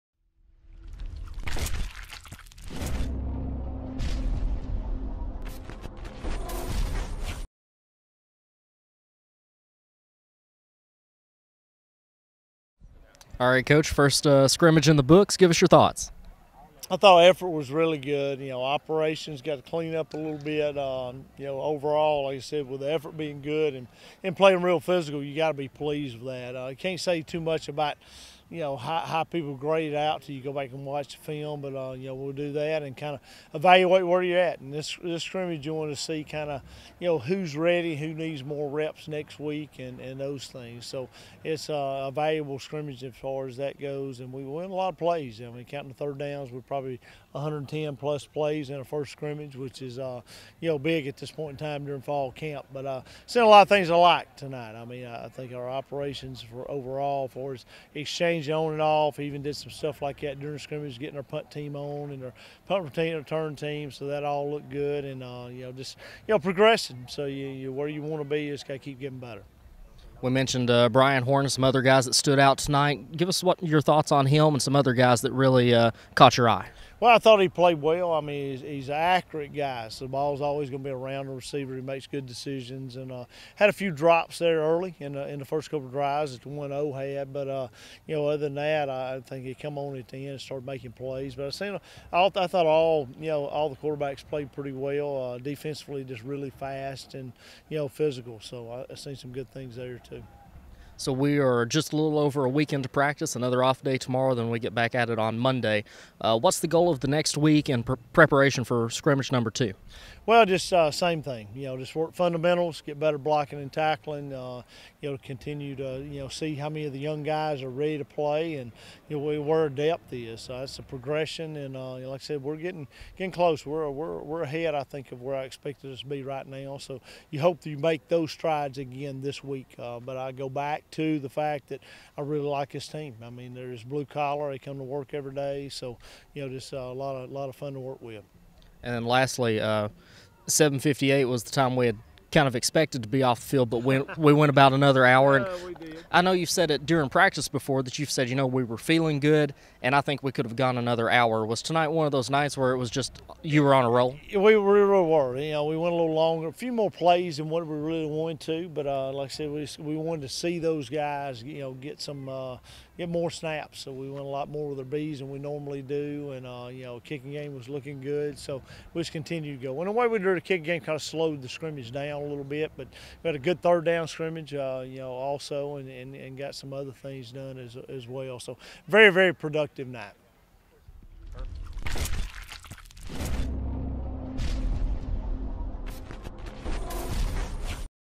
Jacksonville State Football - 2017 Fall Camp - Scrimmage No. 1
Jacksonville State football participates in its first scrimmage of the fall.